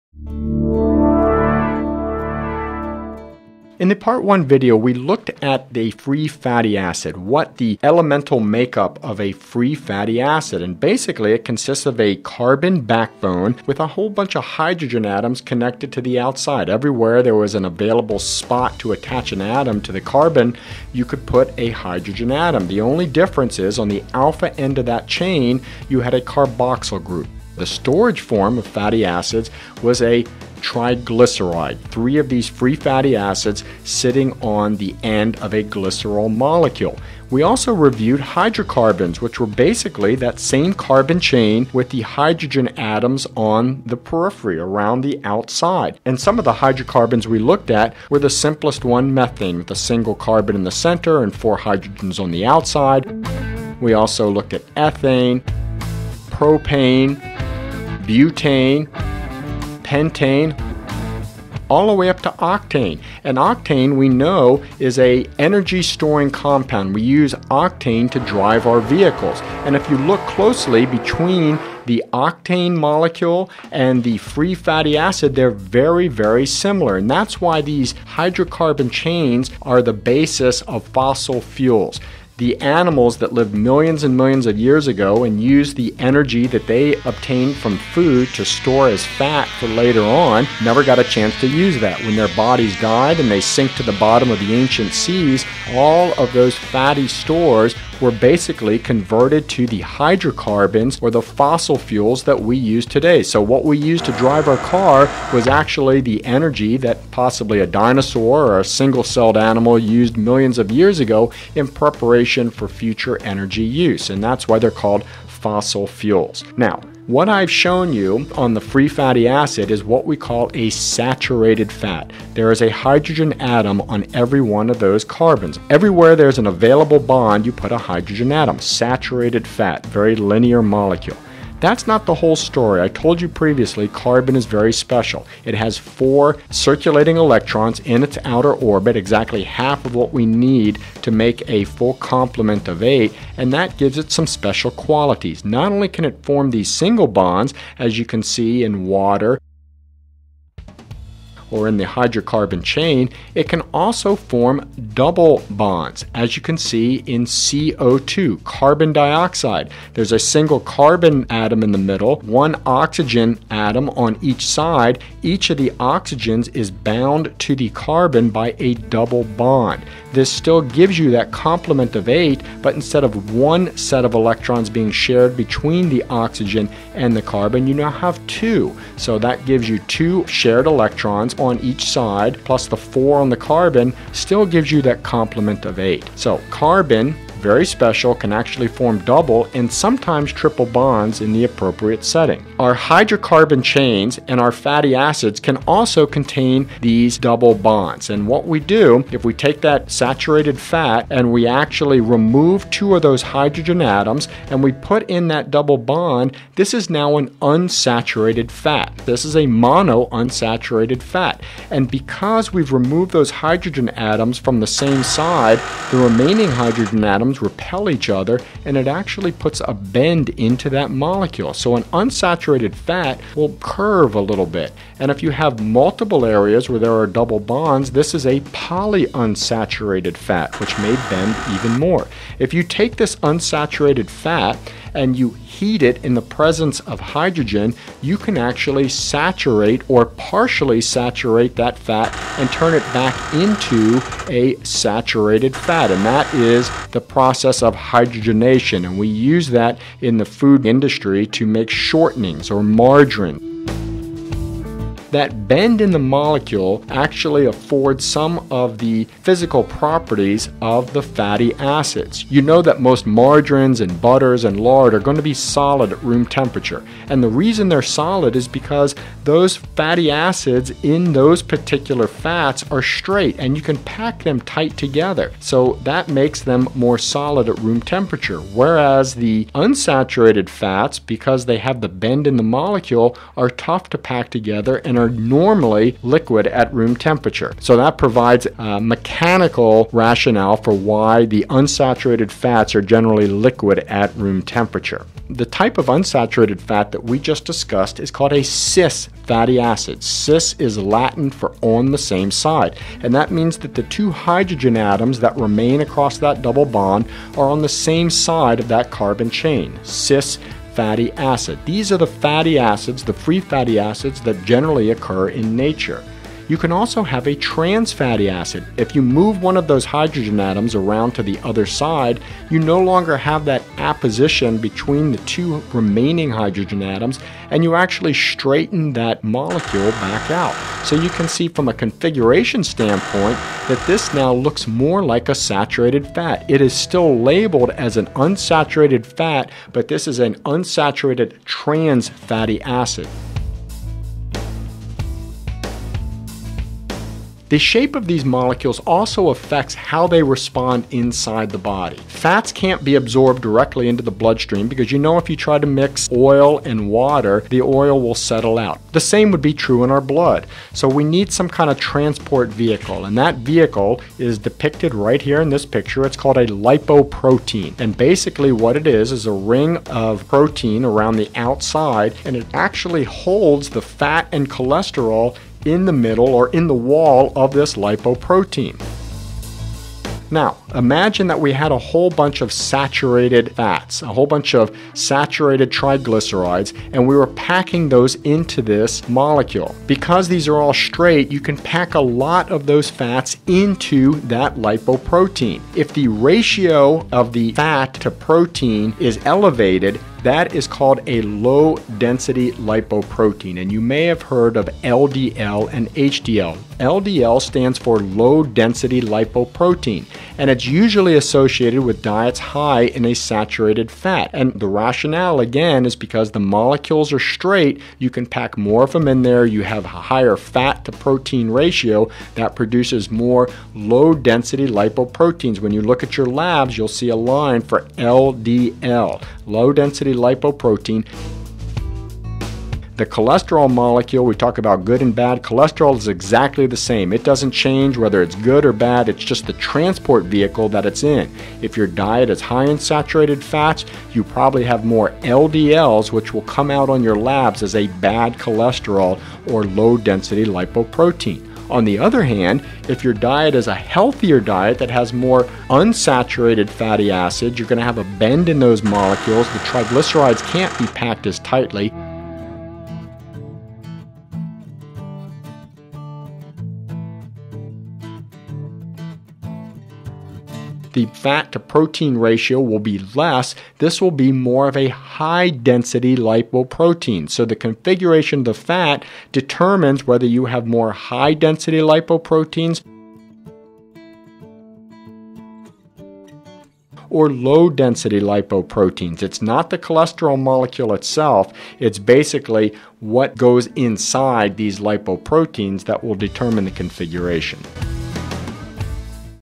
Fat: Part 2 of 4 "Saturated, Unsaturated & Trans fats, Cholesterol & Fossil Fuels"
Describes the molecular and physical differences between a saturated and unsaturated fat and their effect on the fatty transport vehicle, the lipoprotein.  Also describes the relationship of fatty acids, hydrocarbons & fossil fuels as energy storing molecules.